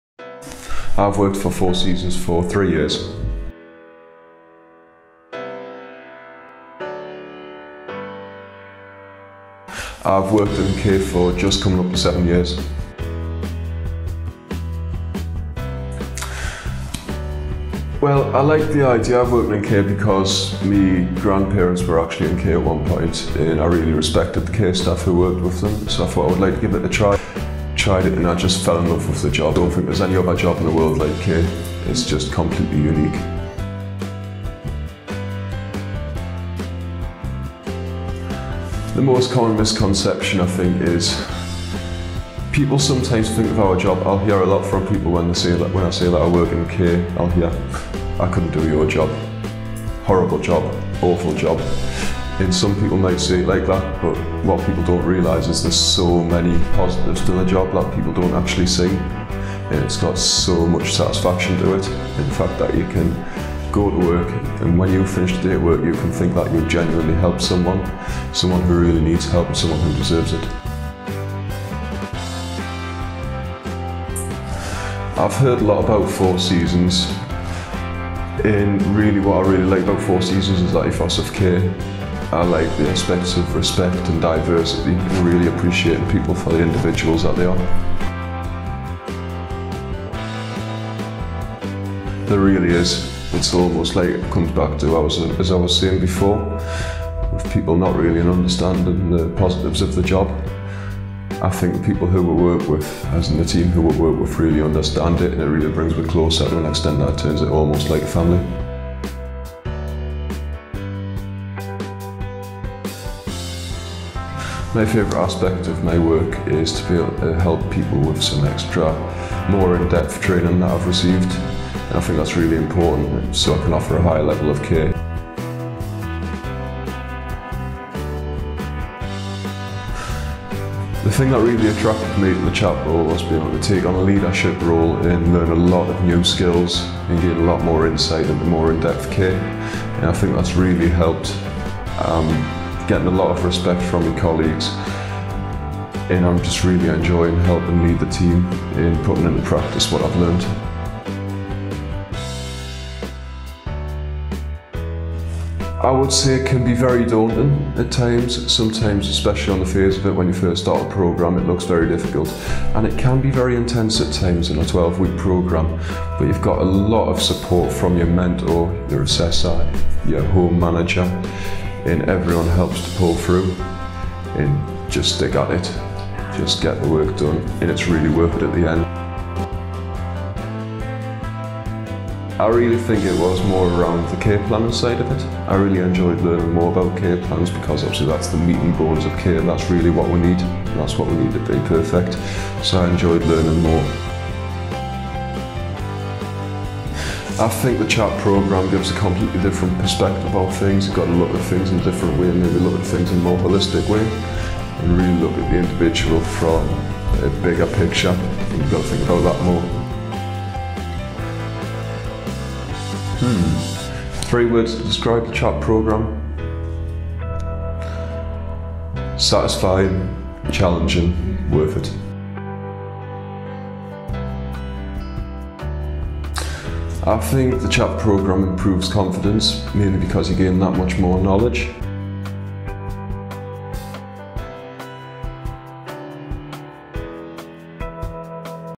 Care Home Assistant Practitioner Programme for Senior Carers | Four Seasons Health Care Careers
Craig has worked for Four Seasons Health Care for 3 years and quickly progressed to a Senior Carer role. In this video, Craig explains how the Care Home Assistant Practitioner (CHAP) Programme allows him to offer a higher level of care and construct more in-depth Care Plans for his residents. Craig also highlights some of the misconceptions about working in elderly health care and explains why he pursued a career in care.